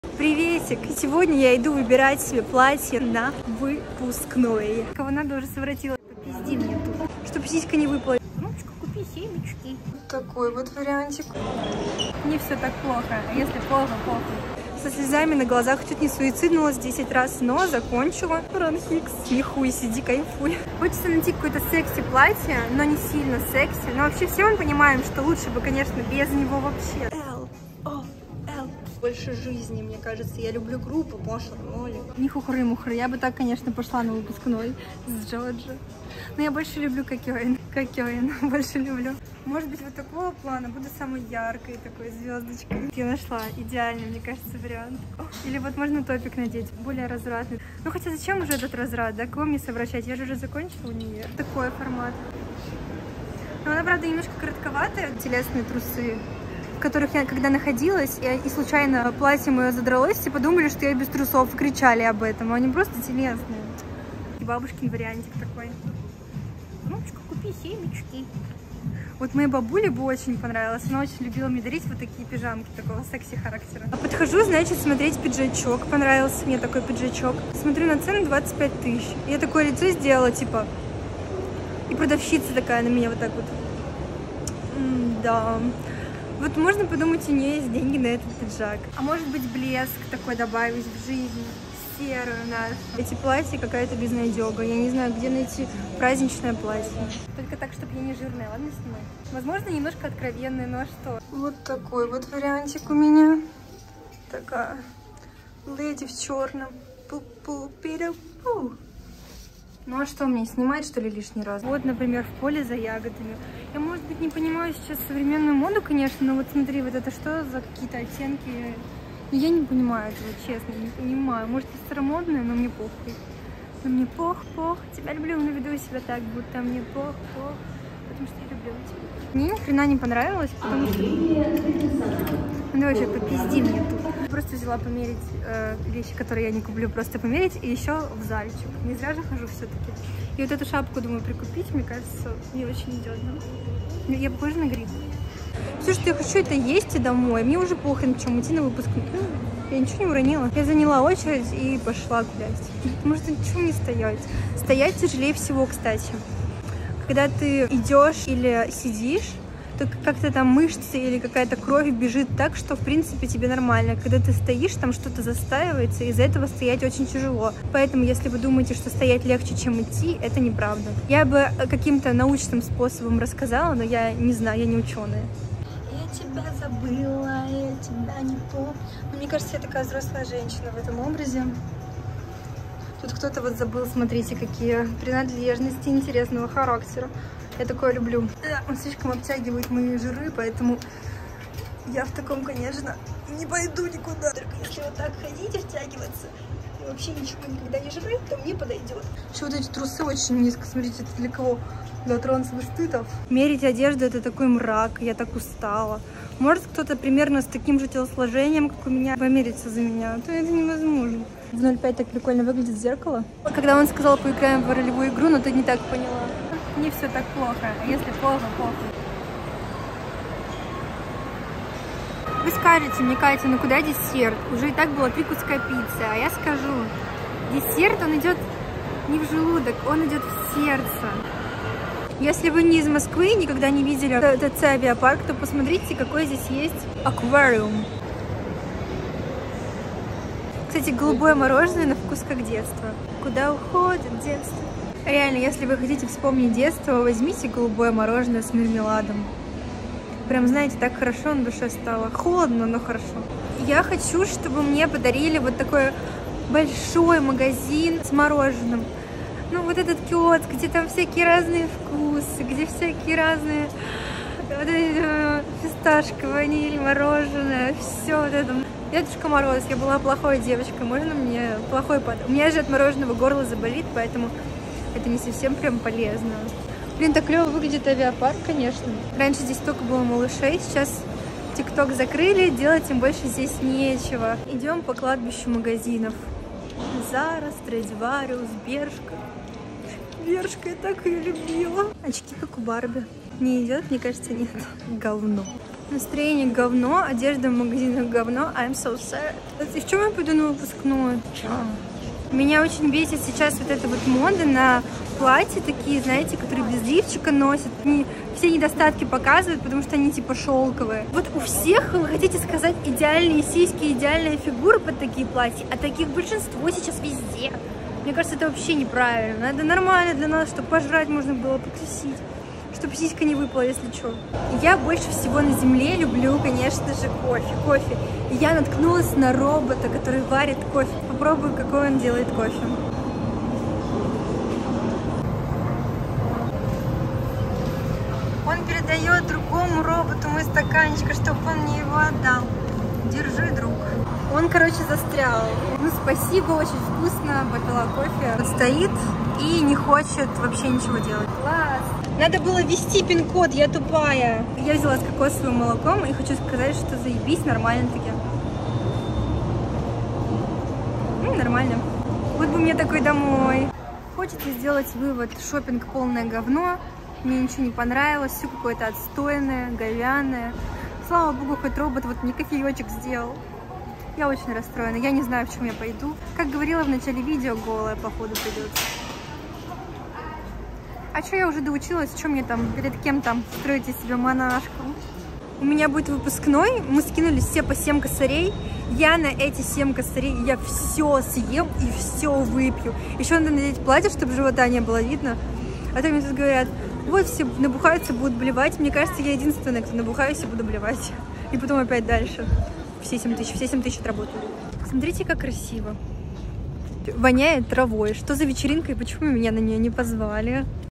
Пойду на выпускной ГОЛАЯ?
Ну и к какому результату приведут мои поиски платья?)

Инста: sunkateworld